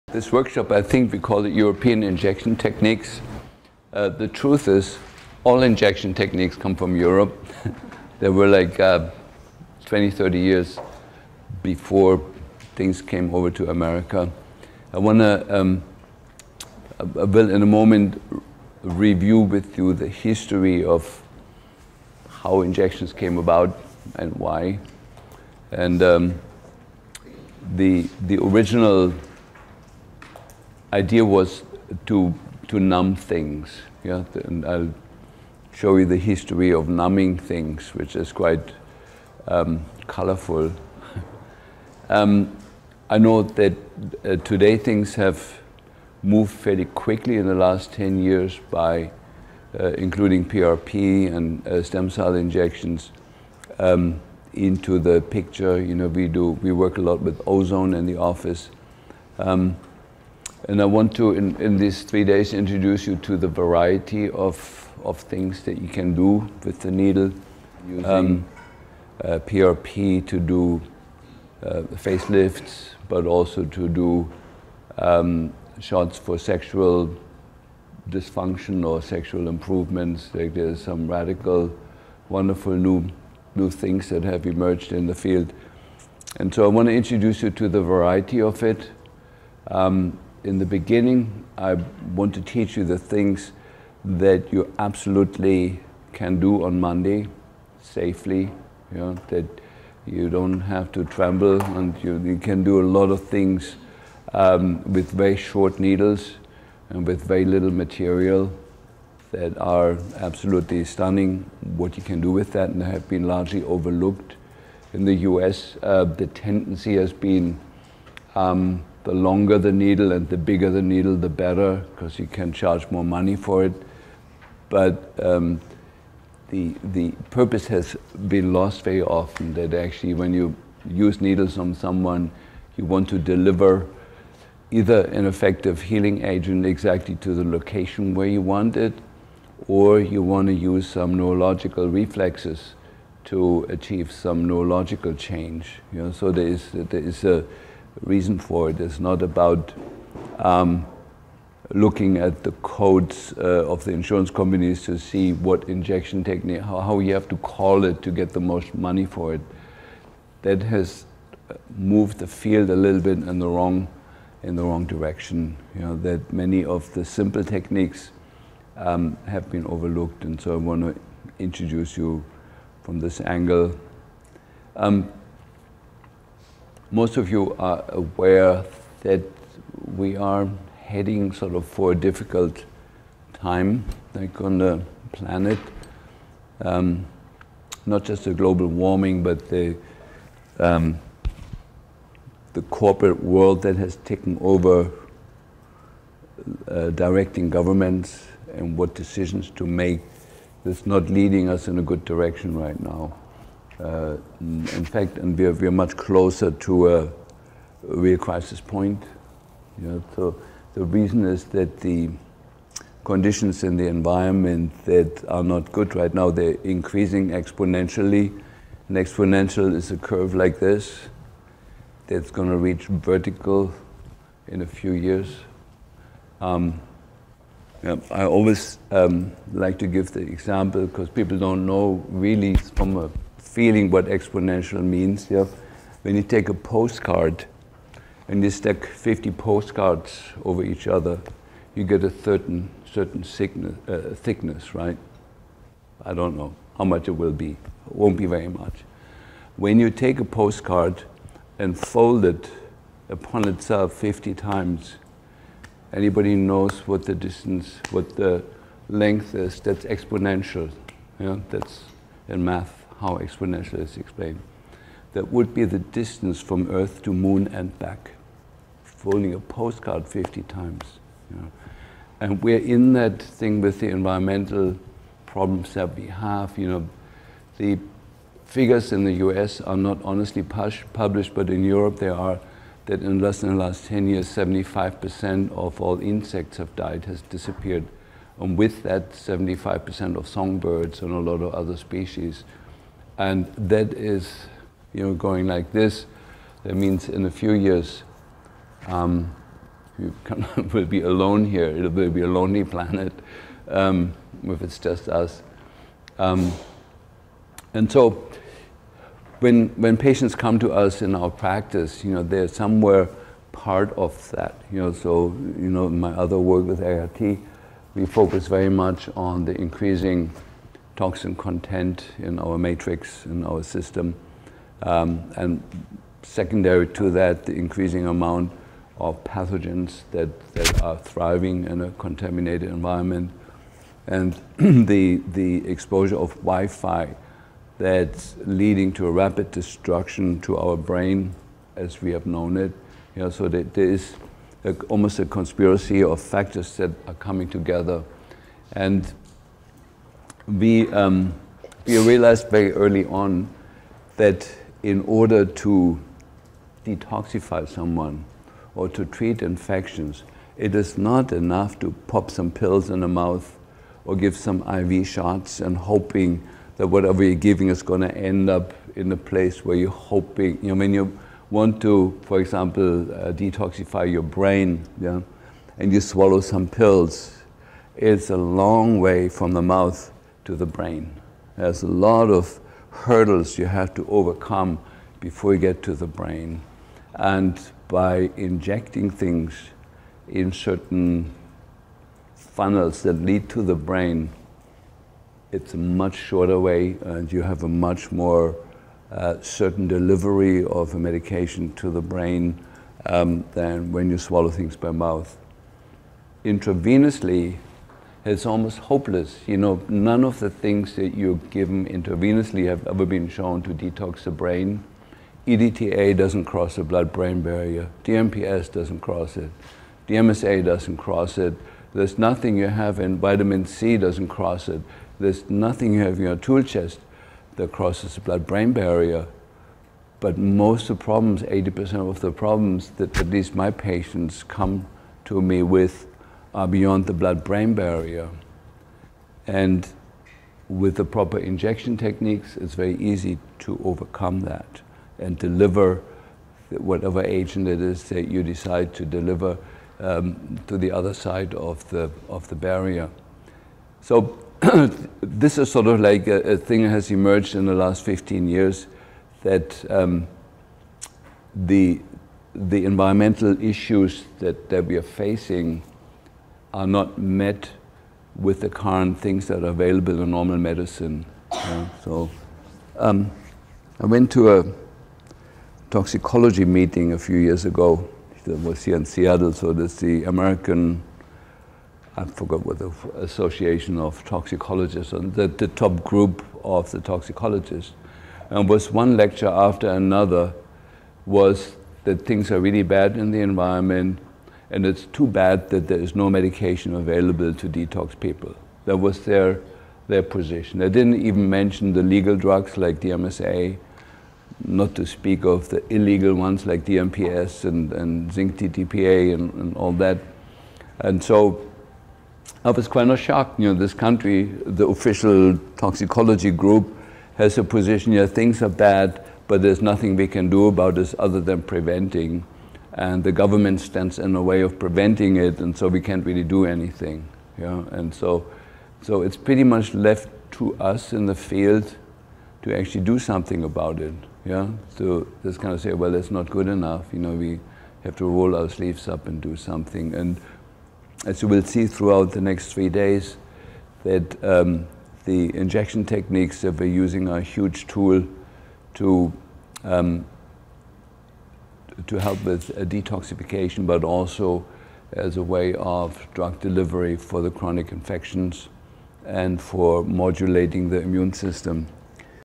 Klinghardt - Mastering Injection Techniques -Neural Therapy- Bee Venom Treatment
Variety of things you can do with the needle, scar injections, easy to incorporate by a practitioner- Neural Therapy.
The newer techniques of interest - PRP, Face Lifts, Lymphatic Neck/Brain drainage, Crown of Thorns, etc. The simple techniques that have been overlooked that you can do immediately in your office with lasting positive effects for your patient. An elegant way to modulate the immune system.
Discussion: toxins, pathogens, wifi exposure